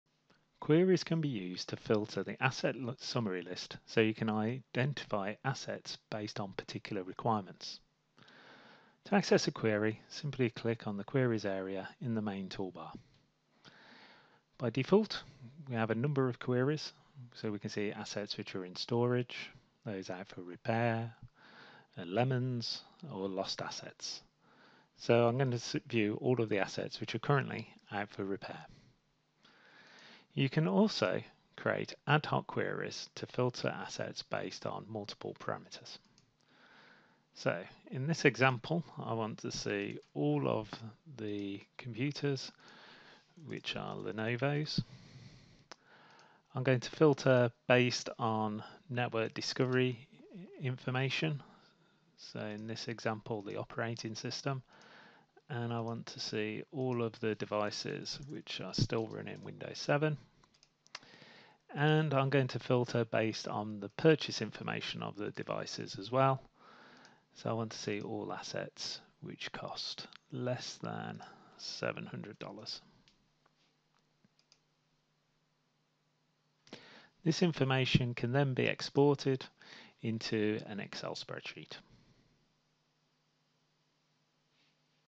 Filter Assets - IT Asset Management Solution - VIZOR
Learn how to filter through assets in VIZOR in this video.